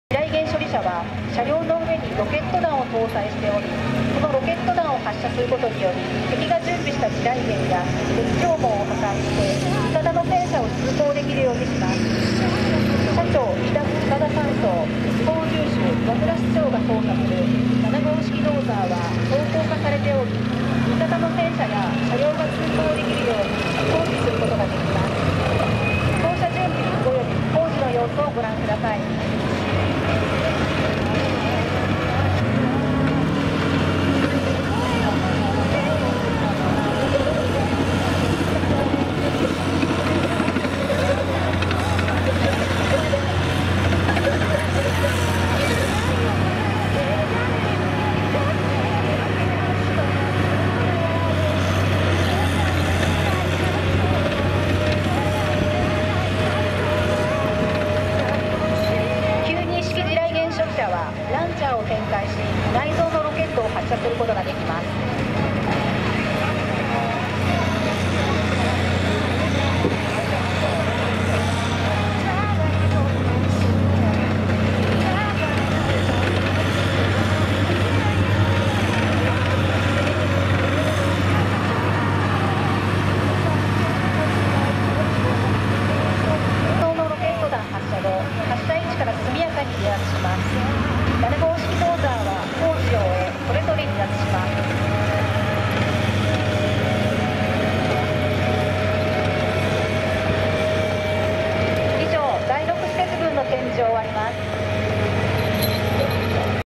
［第6施設群］陸上自衛隊豊川駐屯地 訓練展示［92式地雷原処理車／75式ドーザ］
豊川駐屯地（ Japan Ground Self-Defense Force Camp Toyokawa）は、愛知県豊川市穂ノ原に所在し、第10特科連隊等が駐屯している陸上自衛隊の駐屯地である。 駐屯地司令は、第10特科連隊長が兼務。最寄の演習場は、千両演習場と日吉原演習場。

2年ぶりの開催となった陸上自衛隊豊川駐屯地 創立72周年記念行事に行ってきました。今回は装備品展示編、式典・訓練展示編、第10音楽隊編に分けてご紹介致します。

今回は92式地雷原処理車・75式ドーザの訓練展示の様子をご紹介致します。

［第6施設群］
第6施設群（JGSDF 6th Engineer Group(Construction)）は、愛知県豊川市の豊川駐屯地に主力が駐屯する陸上自衛隊第4施設団隷下の施設科部隊である。

［92式地雷原処理車］
92式地雷原処理車は、広範囲に地雷が敷設された地雷原を啓開することを目的としている陸上自衛隊の車両。1992年に制式採用され、主に施設科・戦車連隊本部管理中隊に配備されている。略称はMBRS、愛称は「マインスィーパー」。

［75式ドーザ］
75式ドーザは、陸上自衛隊の施設科が使用している装甲付のブルドーザーである。製造は小松製作所、愛称はビッグブルの他に、部隊内では「装甲ドーザ」や、単に「ドーザ」とも呼ばれる。

Japan Ground Self-Defense Force Camp Toyokawa is located in Honohara, Toyokawa City, Aichi Prefecture, and is a garrison of the Ground Self-Defense Force where the 10th Special Regiment is stationed. The garrison commander is also the commander of the 10th Special Forces Regiment. The nearest training grounds are Senryo Training Area and Hiyoshihara Training Area.

I went to the 72nd anniversary of the Toyokawa Garrison of the Ground Self-Defense Force, which was held for the first time in two years. This time, we will introduce you to the exhibition of equipment, ceremony and training, and the 10th Music Band.

This time, we would like to introduce the training display of the Type 92 Minefield Disposal Vehicle and Type 75 Dozer.

［6th Facility Group]
The 6th Engineer Group (JGSDF 6th Engineer Group (Construction)) is an installation unit under the 4th Installation Group of the Ground Self-Defense Force, with its main forces stationed at Toyokawa Garrison in Toyokawa City, Aichi Prefecture.

［Type 92 Minefield Clearance Vehicle]
The Type 92 Minefield Clearance Vehicle (MBRS) is a Ground Self-Defense Force vehicle used to clear minefields where mines have been laid over a wide area. Its abbreviated name is MBRS, and its nickname is "Minesweeper.

［Type 75 Dozer]
The Type 75 dozer is an armored bulldozer used by the JGSDF's Facilities Department. In addition to the nickname "Big Bull," it is also called "armored dozer" or simply "dozer" in units.